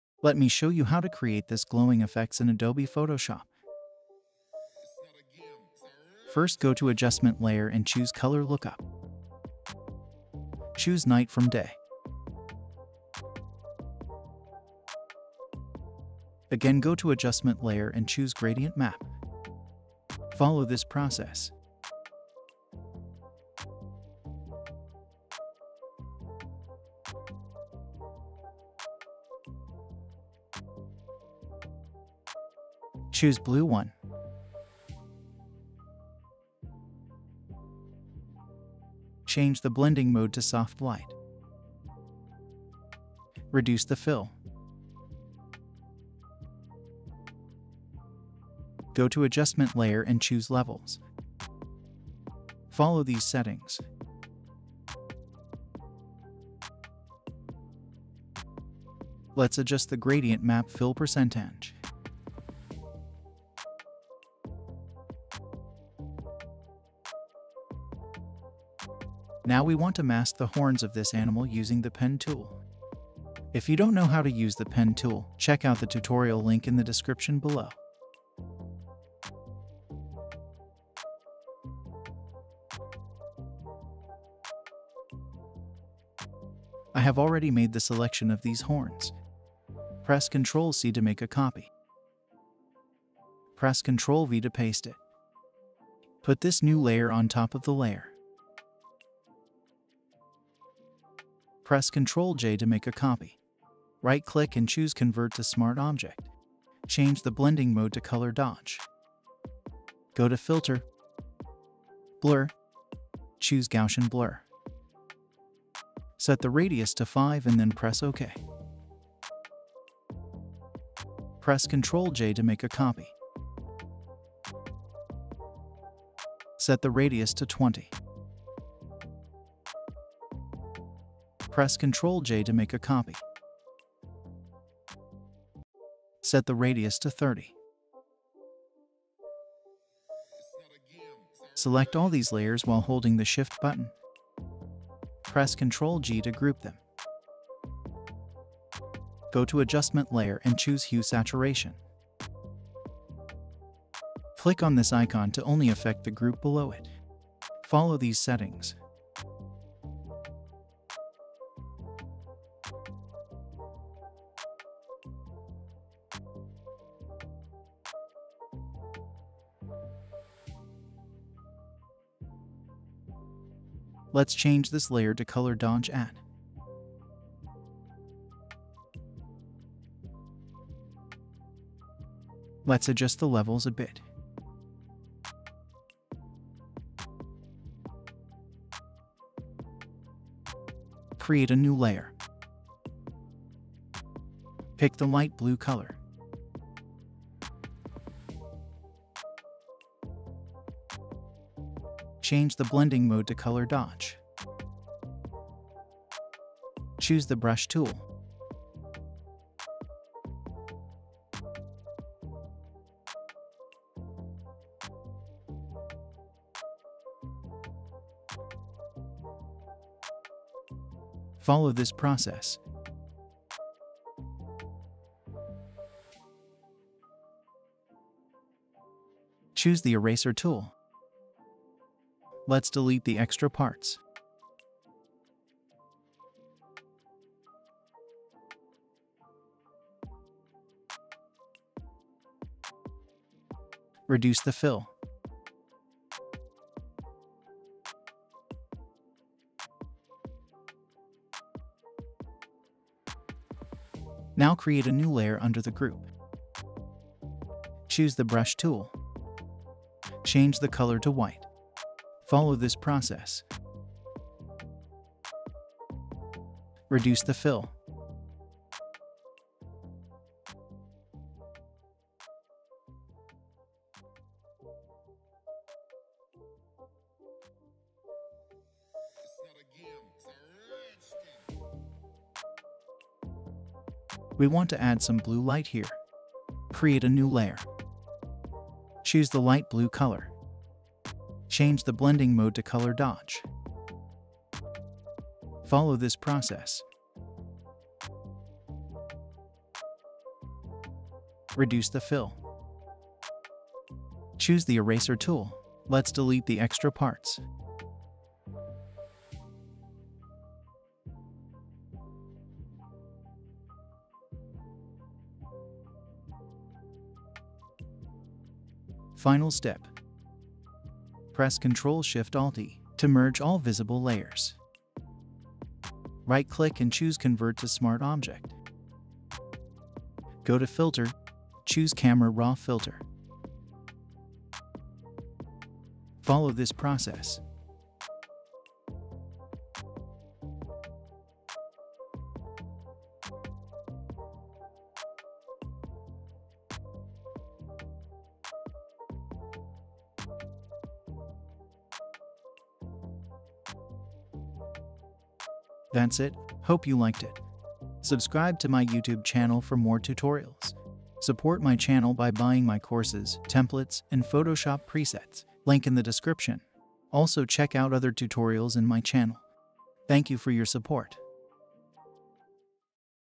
EASY Glowing Effect in Adobe Photoshop
In this tutorial, i will show you how to create a glowing effect in adobe photoshop.

► Design like a Pro using Canva Templates